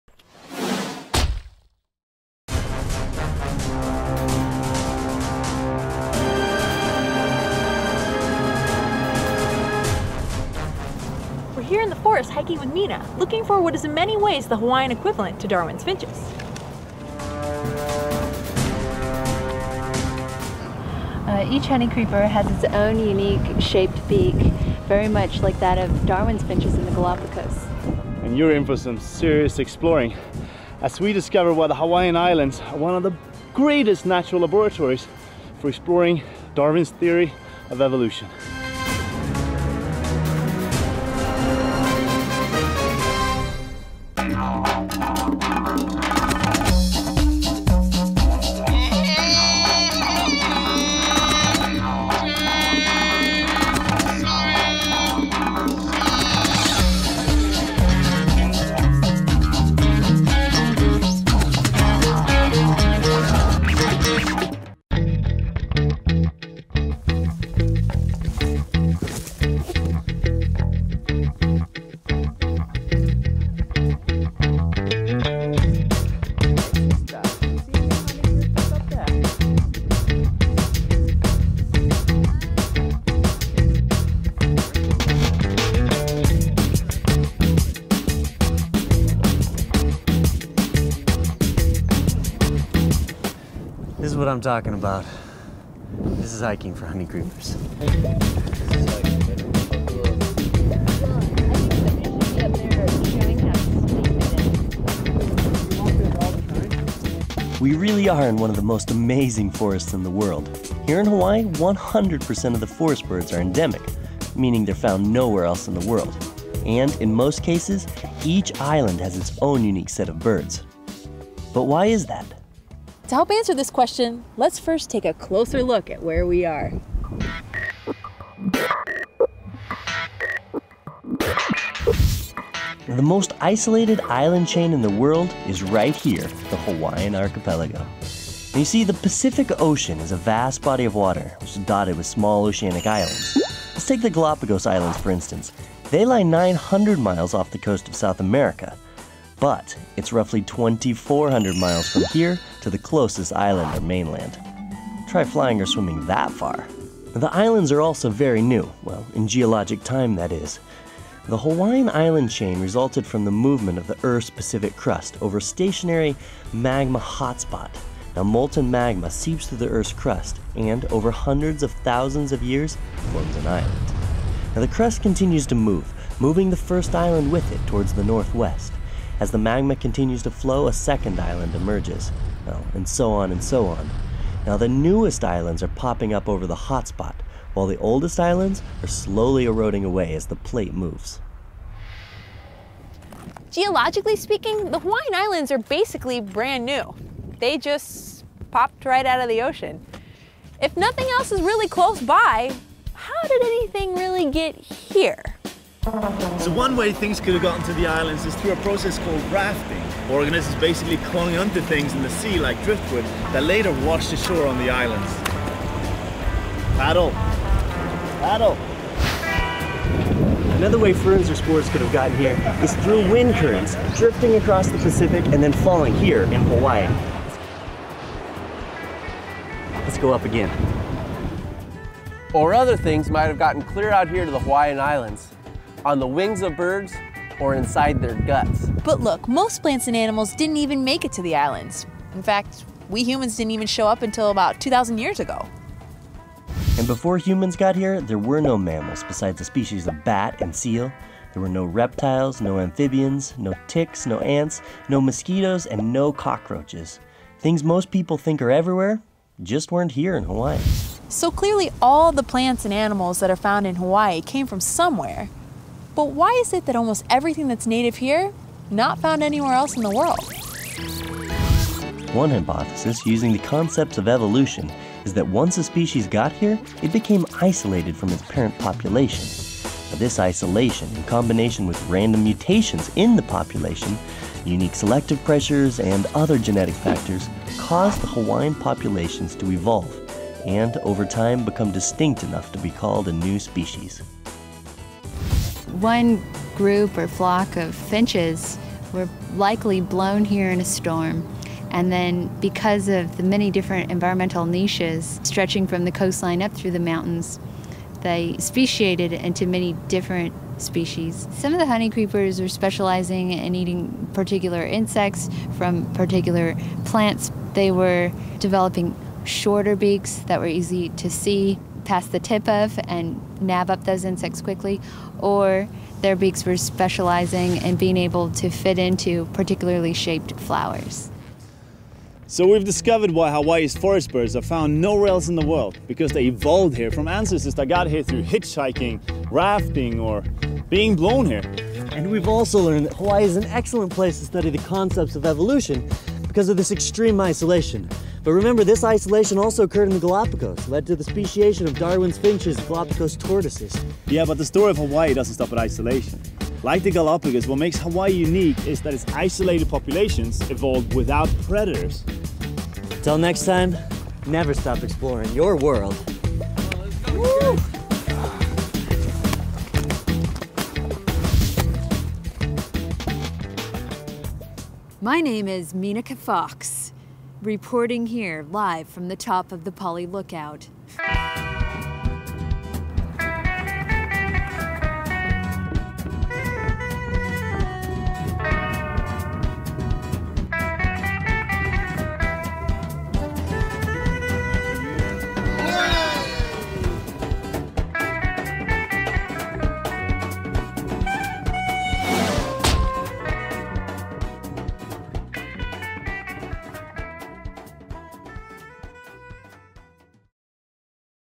Hawaiian Honeycreepers: Evolution in Hawaii
The story of the evolution of Hawaiian Honeycreepers is pretty fascinating. It's been likened to the evolution of Darwin's finches. I spent 5 years in Hawaii while I was in graduate school. My part-time job was to lead people on hikes up the mountains on the island of Hawaii. It allowed me to see some of the native birds in person.

I'm proud to release this video, one we worked on with Pearson Publishing a few years back for their high school biology textbooks. If you don't know a lot about the evolution of these birds, I think you'll enjoy this little retro video from Untamed Science.